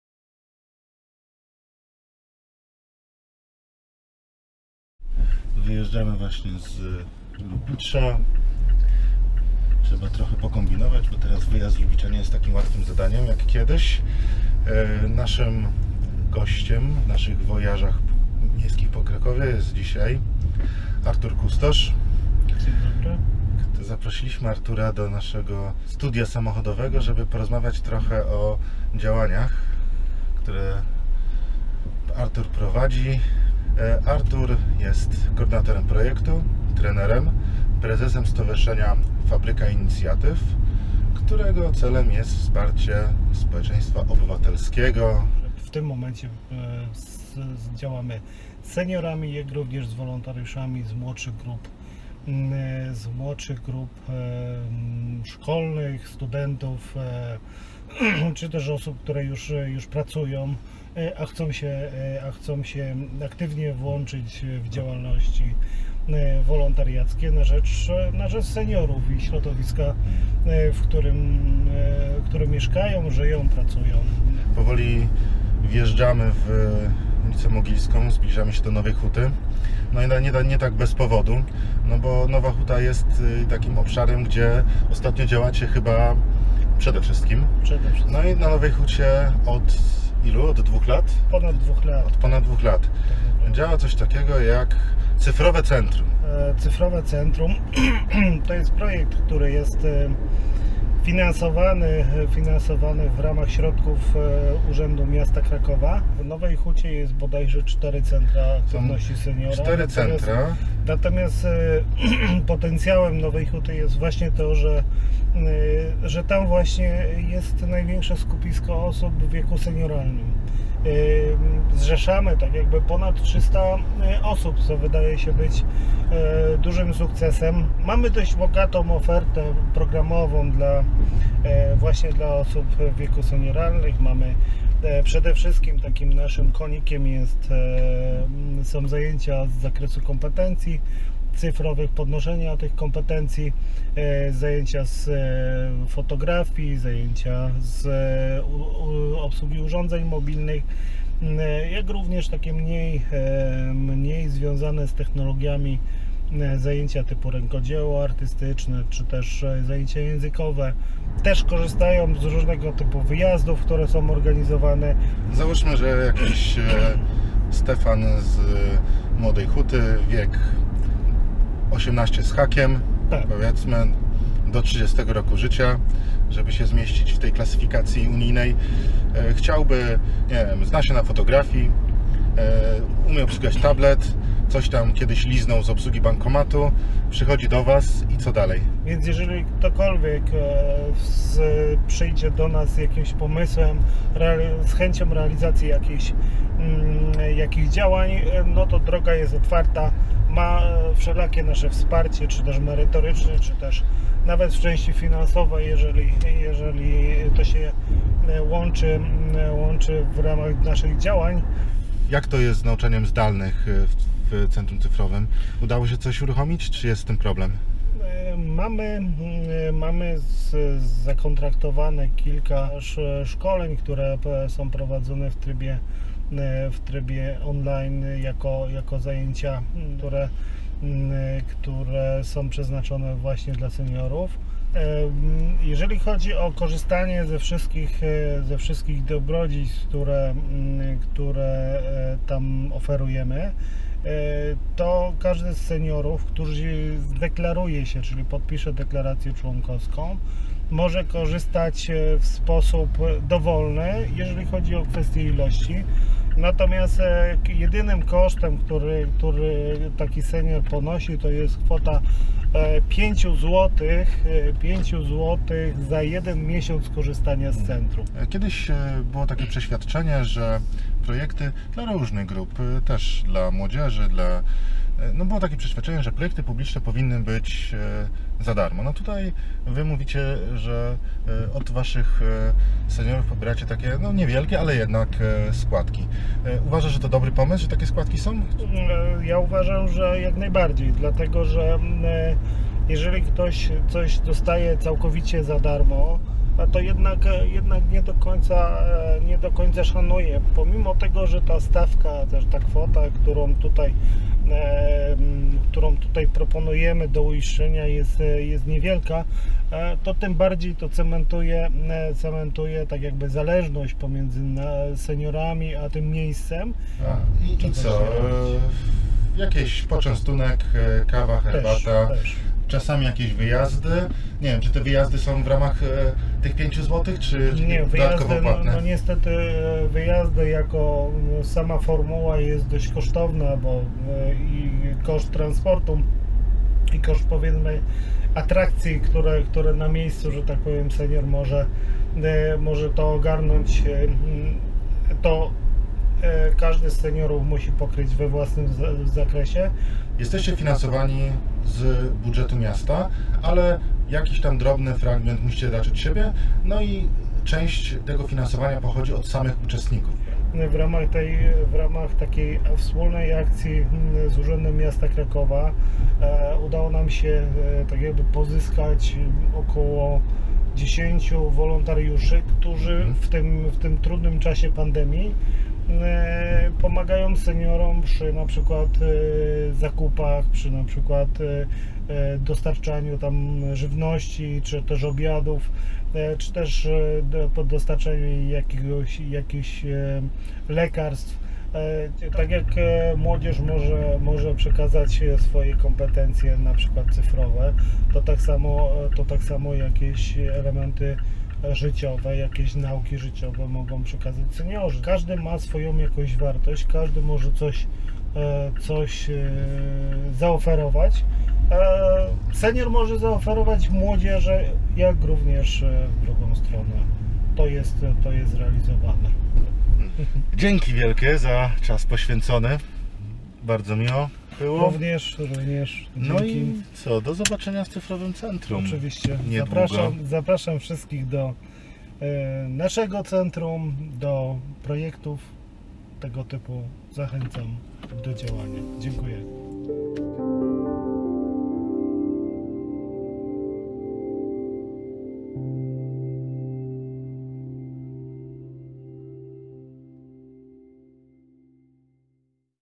Social Innovation Drive - Wywiad z Arturem Kustoszem / Interview with Artur Kustosz
Seria wywiadów z przedstawicielami podmiotów ekonomii społecznej pn. Social Innovation Drive powstała w ramach projektu "Young Social Innovators - Engine of Social Change" dofinansowanego z programu Erasmus+